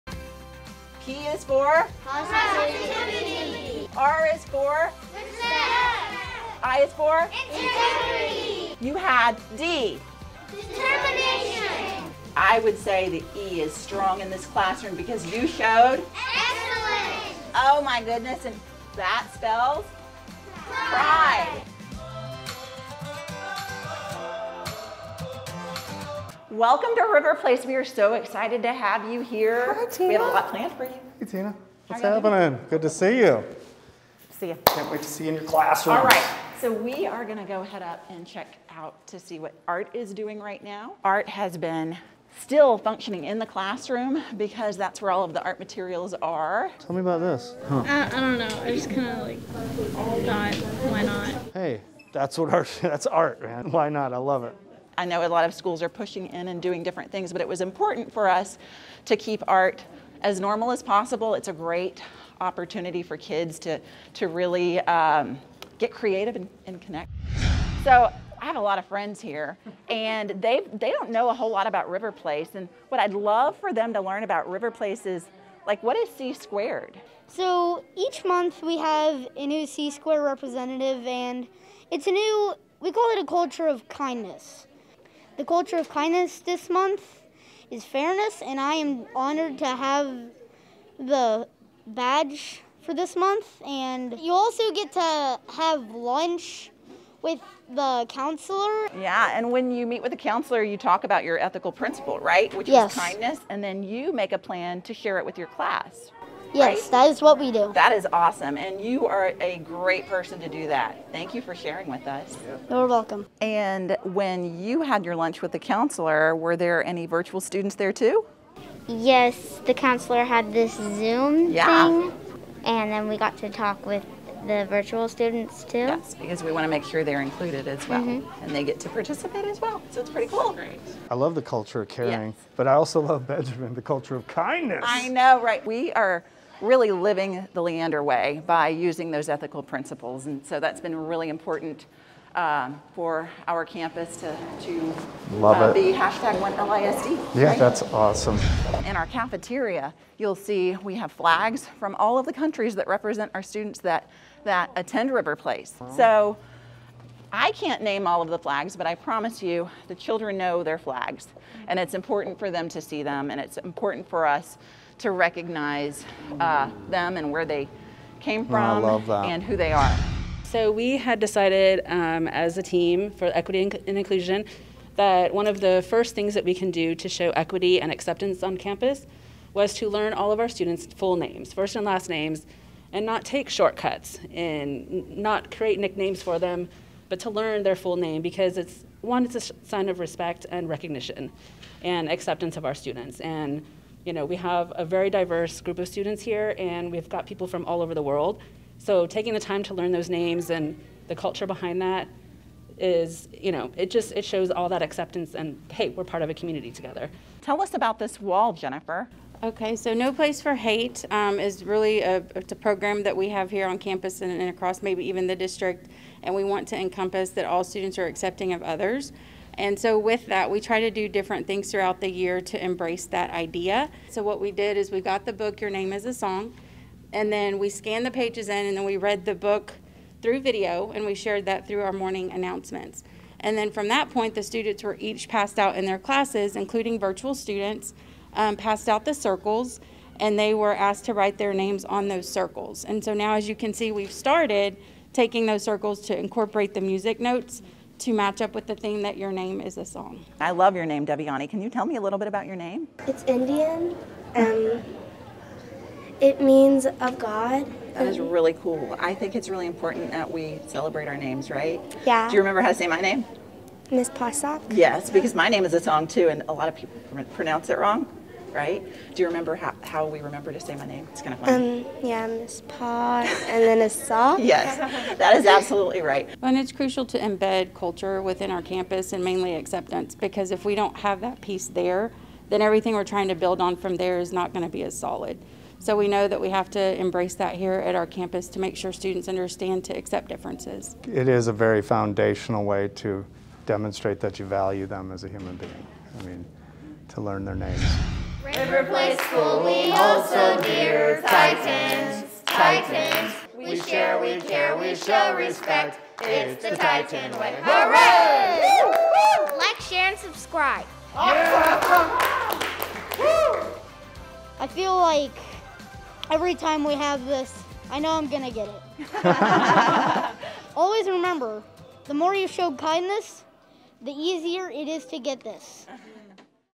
Super School Visit: River Place Elementary
The learning happening at River Place Elementary is more than just the basics. During their Super School Visit, Chief Academic Officer Matt Bentz, Ed.D. and Asst. Superintendent of Curriculum Jennifer Collins witness a vibrant display of school pride. RPE Titans have created a culture of caring and inclusivity. Don’t miss the unique opportunities they’re doing to celebrate every person!